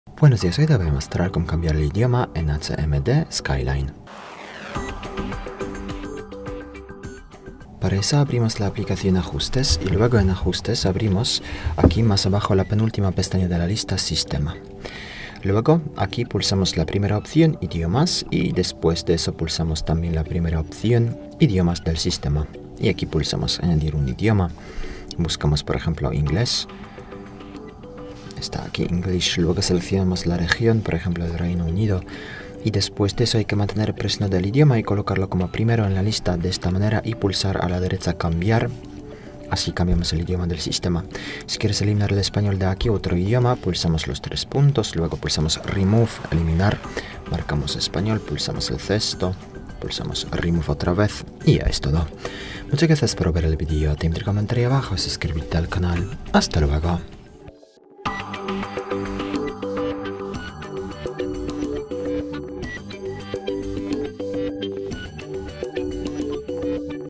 Cómo cambiar el idioma en HMD Skyline
En este video, te mostramos cómo cambiar el idioma en tu HMD Skyline para adaptar el dispositivo a tus preferencias lingüísticas. Aprende a seleccionar un nuevo idioma para el sistema operativo y las aplicaciones de tu dispositivo.

¿Cómo ajustar el idioma del sistema en HMD Skyline?
¿Dónde encontrar la opción para cambiar el idioma en HMD Skyline?
¿Cómo seleccionar un nuevo idioma en HMD Skyline?
¿Cómo cambiar el idioma de las aplicaciones en HMD Skyline?